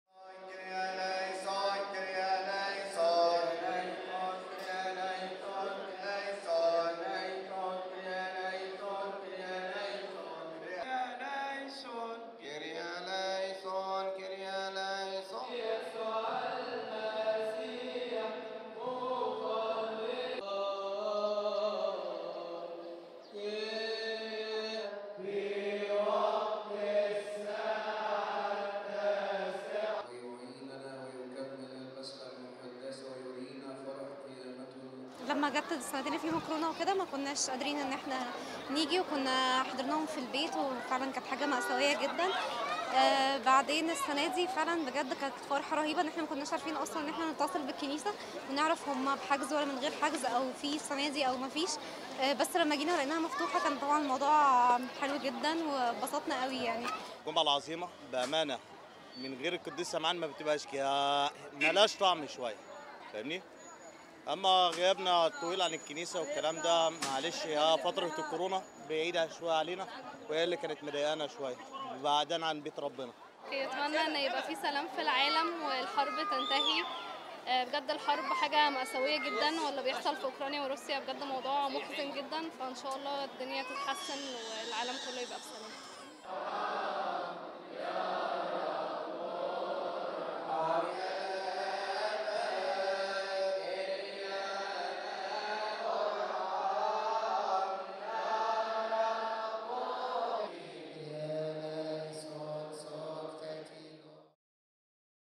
Des chrétiens coptes orthodoxes d'Égypte célèbrent le Vendredi Saint | AFP
Des chrétiens coptes orthodoxes observent les prières du Vendredi Saint au monastère de Saint-Simon, connu sous le nom d'église de la Grotte, dans la montagne de Mokattam, près de la capitale égyptienne.